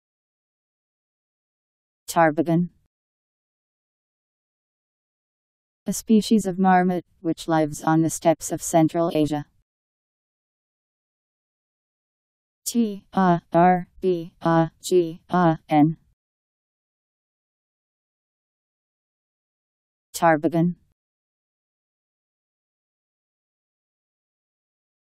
Tarbagan Meaning
Video shows what tarbagan means. A species of marmot, , which lives on the steppes of Central Asia..  Tarbagan Meaning. How to pronounce, definition audio dictionary. How to say tarbagan. Powered by MaryTTS, Wiktionary